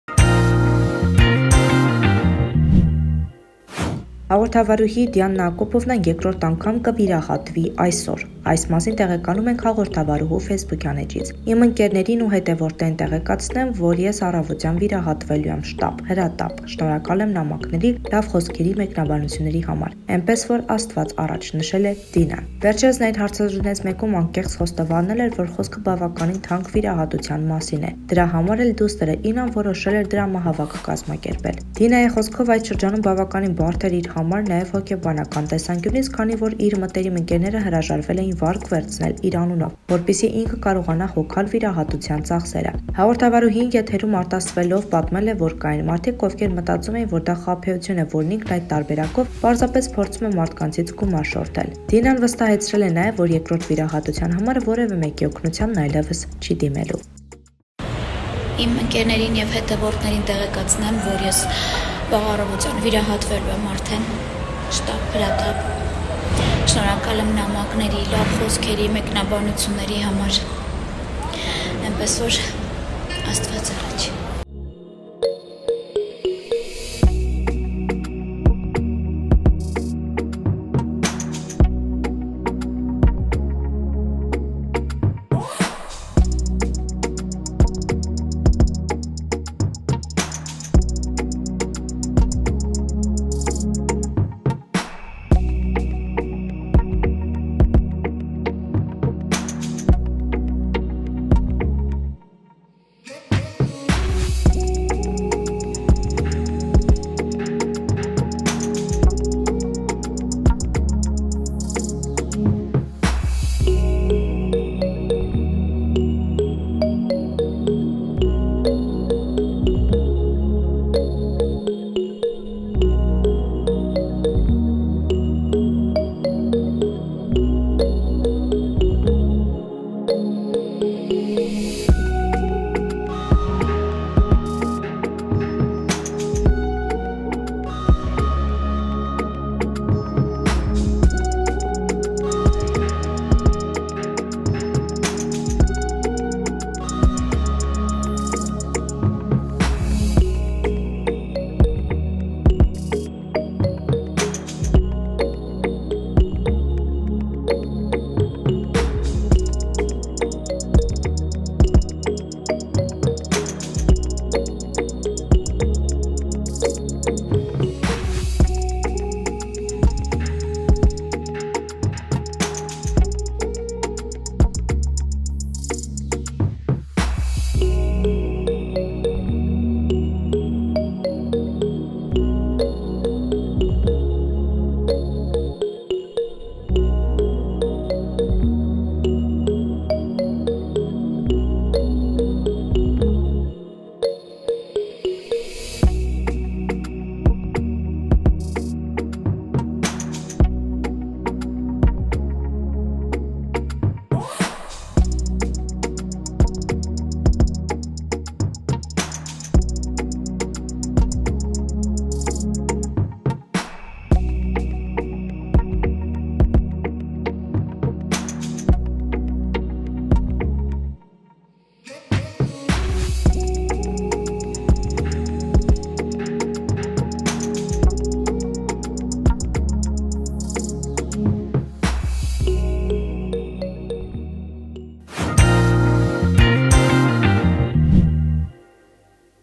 Դինա Ակոպովնան շտապ վիրահատվում է
Հաղորդավարուհի Դինա Ակոպովնան 2-րդ անգամ անգամ կվիրահատվի այսօր։ Այս մասին տեղեկանում ենք հաղորդավարուհու ֆեյսբուքյան էջից։

Մանրամասն՝ տեսանյութում